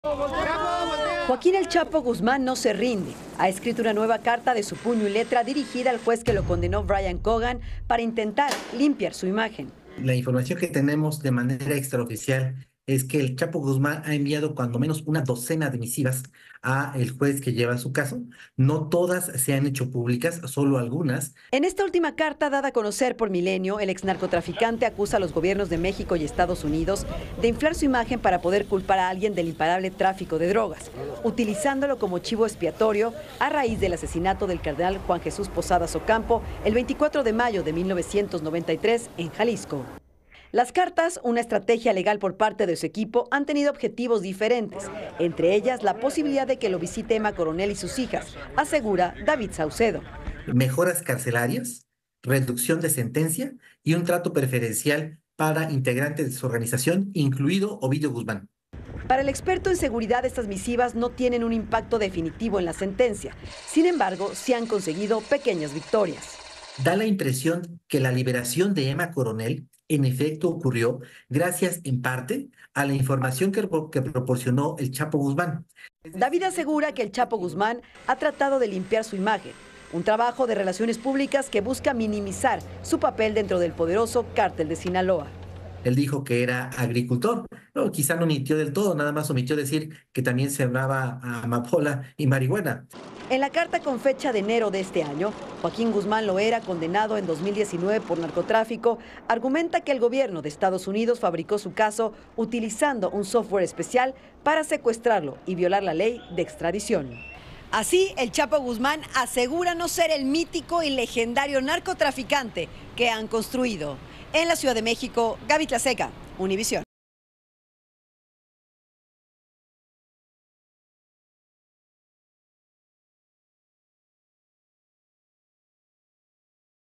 El ‘Chapo’ Guzmán trata de limpiar su imagen: acusa a EEUU y México de usarlo como chivo expiatorio
Joaquín “El Chapo” Guzmán envío una nueva carta al juez que lo condenó a cadena perpetua en la que trata de limpiar su imagen. El exlíder del Cartel de Sinaloa acusó a los gobiernos de EEUU y México de inflar su imagen para tener a quien culpar por el imparable tráfico de drogas.

Maity Interiano